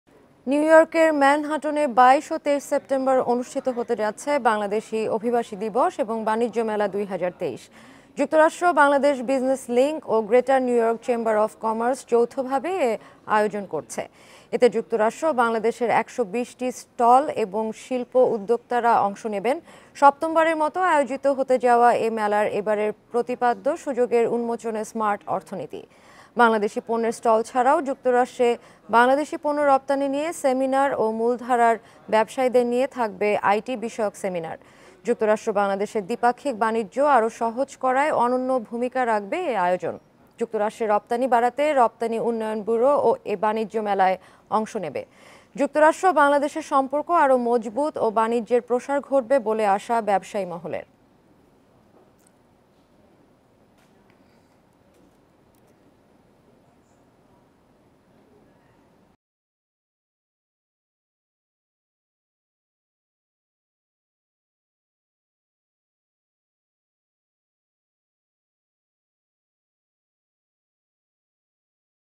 যুক্তরাষ্ট্র ও বাংলাদেশের ১২০টি স্টল ও শিল্প উদ্যোক্তারা অংশ নেবেন || USA | New York | Trade Fair
নিউইয়র্কের ম্যানহাটনে ২২ ও ২৩ সেপ্টেম্বর অনুষ্ঠিত হতে যাচ্ছে বাংলাদেশি অভিবাসী দিবস ও বাণিজ্য মেলা-২০২৩...। যুক্তরাষ্ট্র-বাংলাদেশ বিজনেস লিংক ও গ্রেটার নিউইয়র্ক চেম্বার অব কমার্স যৌথভাবে এ আয়োজন করছে।